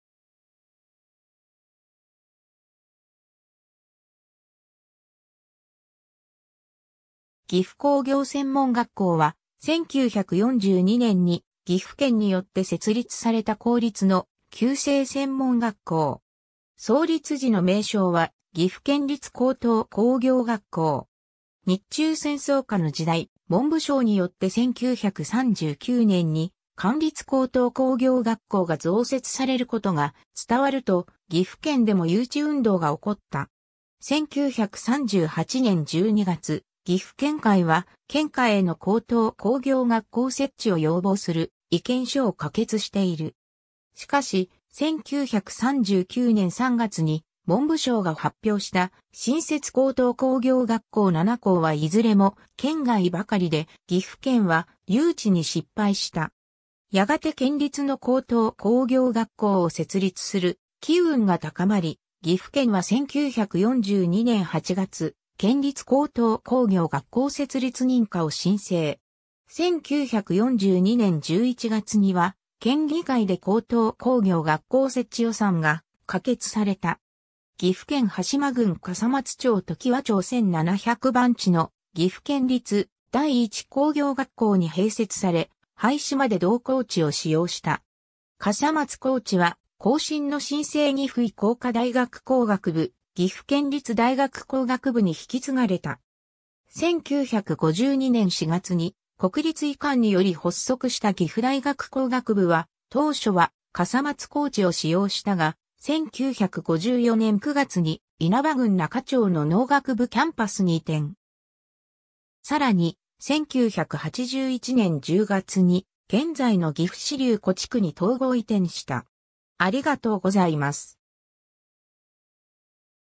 岐阜工業専門学校 (旧制)
岐阜工業専門学校 （ぎふこうぎょうせんもんがっこう） は、1942年 （昭和17年） に岐阜県によって設立された公立の旧制専門学校。
創立時の名称は 「岐阜県立高等工業学校」。
 日中戦争下の時代、文部省によって1939年に官立高等工業学校が増設されることが伝わると、岐阜県でも誘致運動が起こった。
1938年12月、岐阜県会は県下への高等工業学校設置を要望する意見書を可決している。
しかし、1939年3月に文部省が発表した新設高等工業学校 7校はいずれも県外ばかりで、岐阜県は誘致に失敗した。
やがて県立の高等工業学校を設立する機運が高まり、岐阜県は1942年8月、県立高等工業学校設立認可を申請。
1942年11月には、県議会で高等工業学校設置予算が可決された。
 岐阜県羽島郡笠松町常盤町1700番地の岐阜県立第一工業学校 （現・岐阜県立岐阜工業高等学校） に併設され、廃止まで同校地を使用した。
笠松校地は後身の新制岐阜医工科大学工学部・岐阜県立大学工学部に引き継がれた （当時 笠松校地に同居していた岐阜県工業試験場の一部研究室も、大学附属研究室とされた）。
1952年4月に国立移管により発足した岐阜大学工学部は、当初は笠松校地を使用したが、1954年9月に稲葉郡那加町 （現・各務原市那加門前町） の農学部キャンパス （元・旧制岐阜高等農林学校校地） に移転。
さらに、1981年10月に、現在の岐阜市柳戸地区に統合移転した。